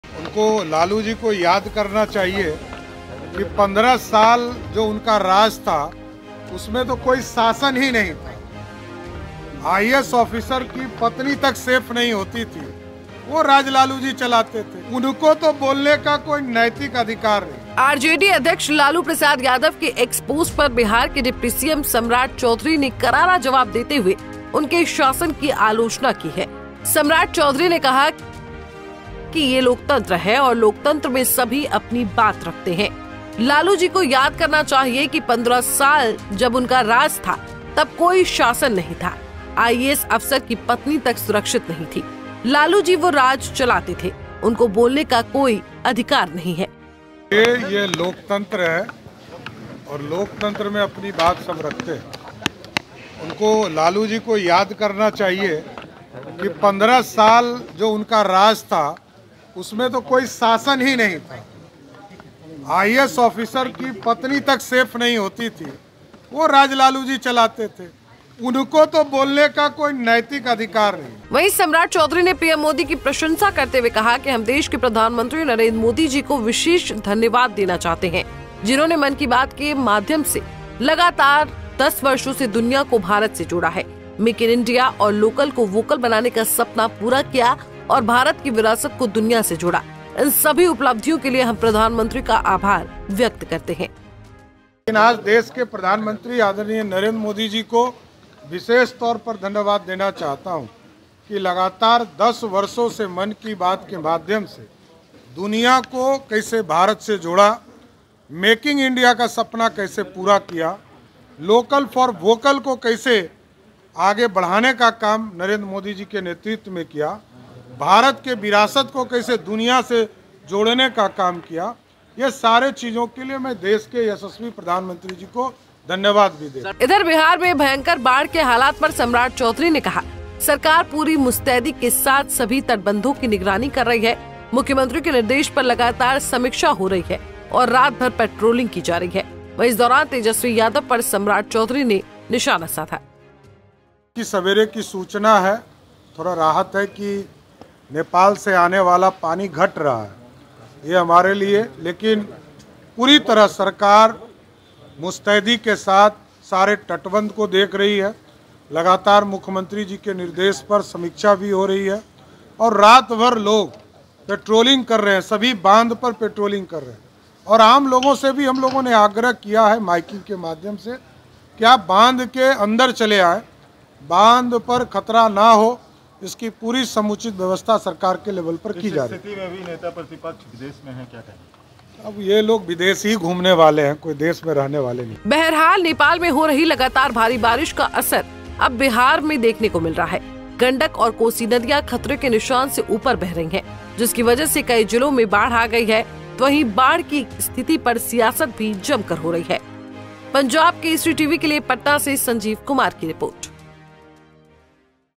‘IAS अफसर की पत्नी तक सुरक्षित नहीं थी’, Lalu Yadav के X पोस्ट पर Samrat Choudhary तगड़ा जवाब | NDA
Bihar Political News: RJD अध्यक्ष लालू प्रसाद यादव ( Lalu Yadav ) के X पोस्ट पर बिहार के डिप्टी सीएम सम्राट चौधरी ( Samrat Choudhary ) ने करारा जवाब देते हुए उनके शासन की आलोचना की है. सम्राट चौधरी ने कहा कि, यह लोकतंत्र है और लोकतंत्र में सभी अपनी बात रखते हैं… लालू जी को याद करना चाहिए कि 15 साल जब उनका राज था तब कोई शासन नहीं था, IAS अफसर की पत्नी तक सुरक्षित नहीं थी…. लालू जी वह राज चलाते थे, उनको बोलने का कोई अधिकार नहीं है…..